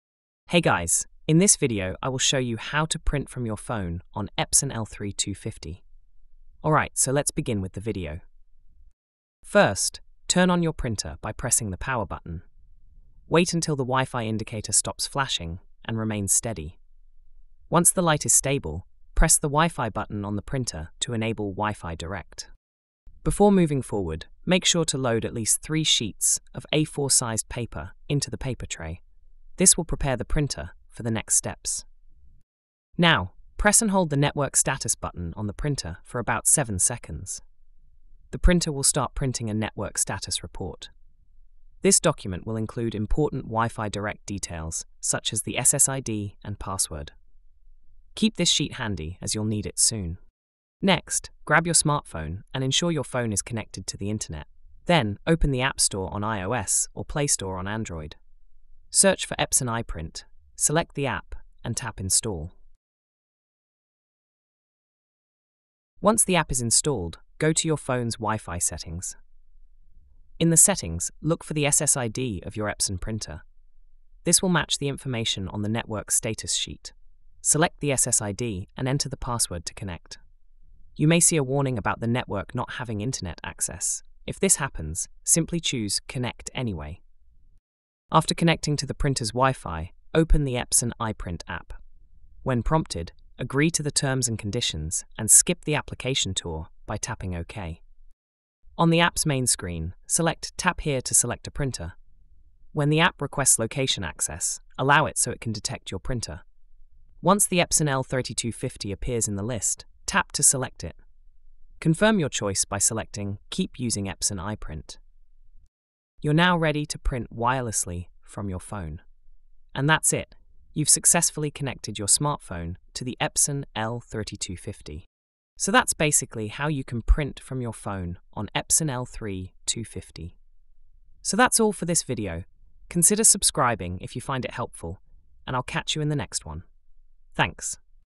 How To Print From Your Phone On Epson L3250
In this video, I’ll show you how to print directly from your phone on the Epson L3250 printer. Whether you’re using Android or iPhone, I’ll guide you step by step through the easiest method using the Epson Smart Panel app. Make sure your printer and phone are on the same Wi-Fi network for a smooth connection.